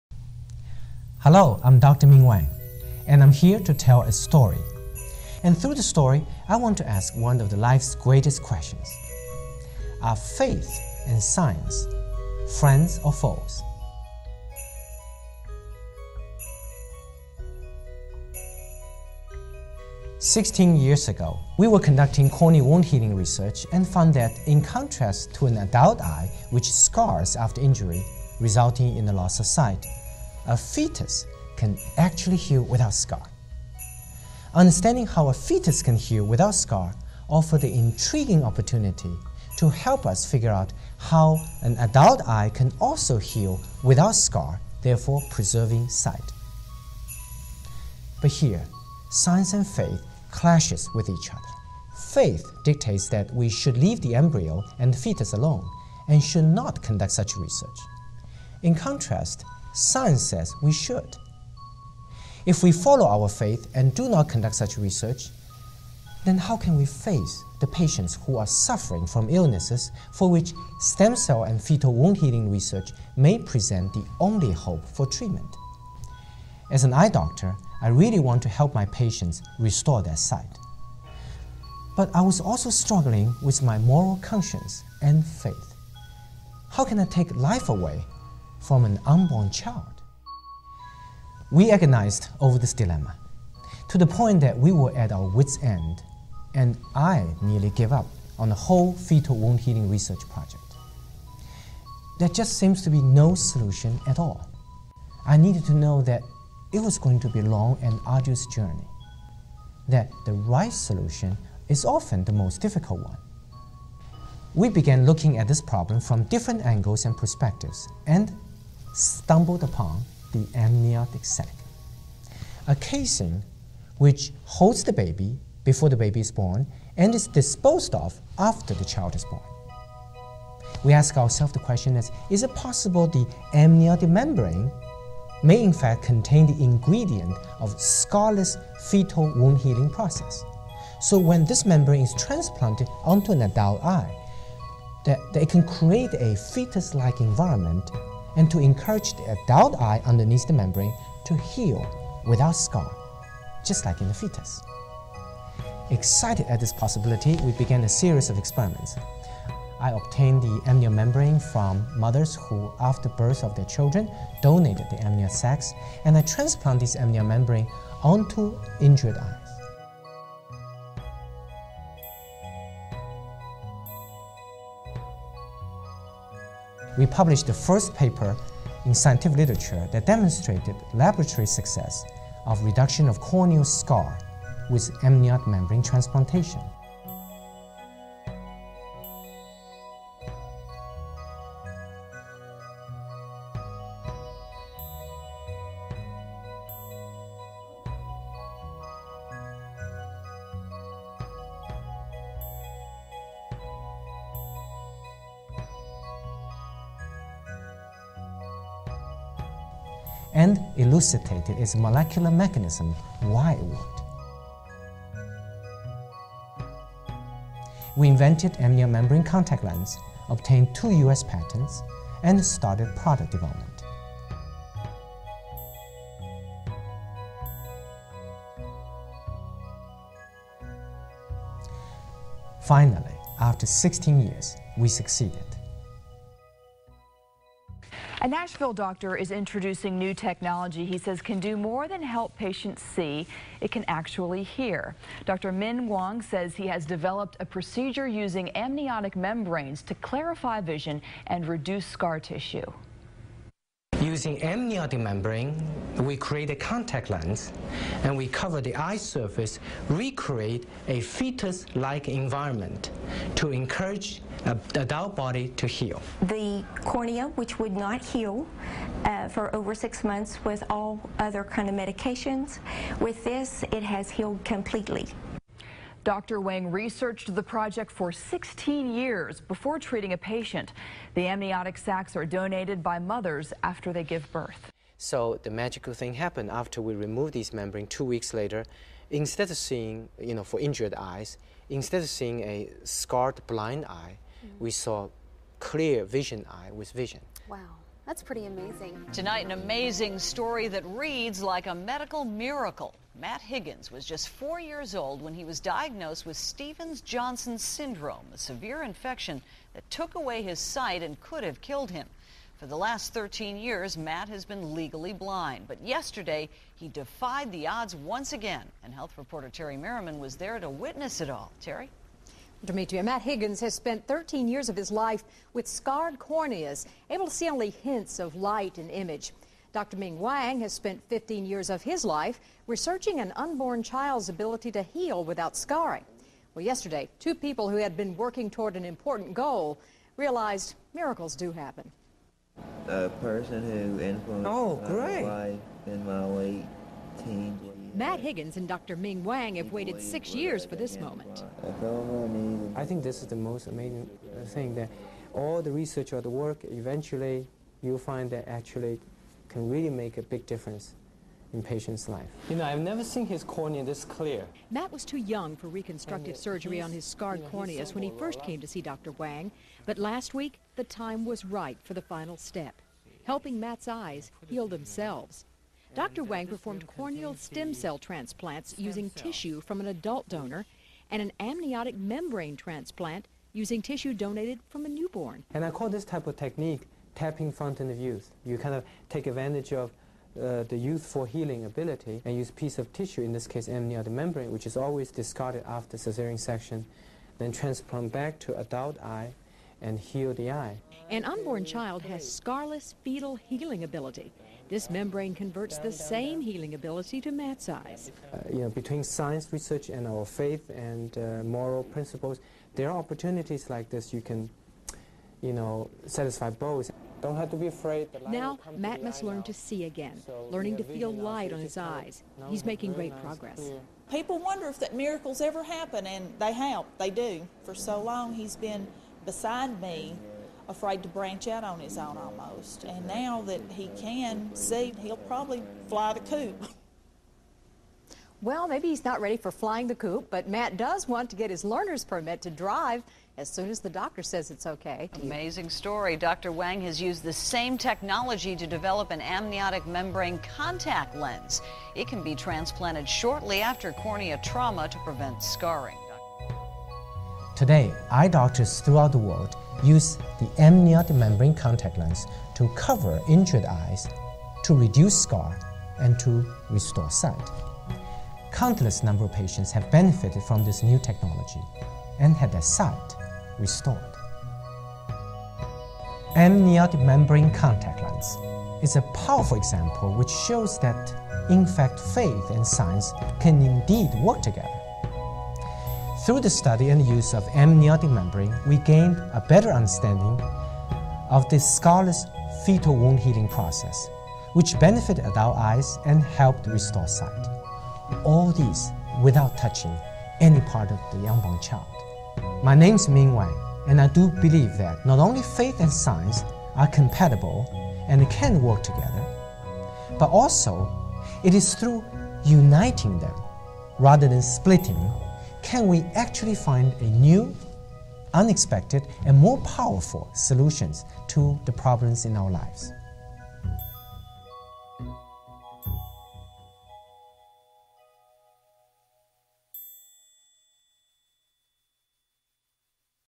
Amniotic membrane contact lens (AMCL) Story
AMCL takes advantage of the youthful healing property of amniotic membrane to help adult eye heal after injury. It is based on two U.S patents by Dr. Wang and Mr. Chris Adams. The AMCL product is available now, through IOP, Inc. Since the AmbioDisk AMCL was launched in Nov 2011, over 500 U.S. eye doctors have used it and have treated many patients with poor ocular surfaces.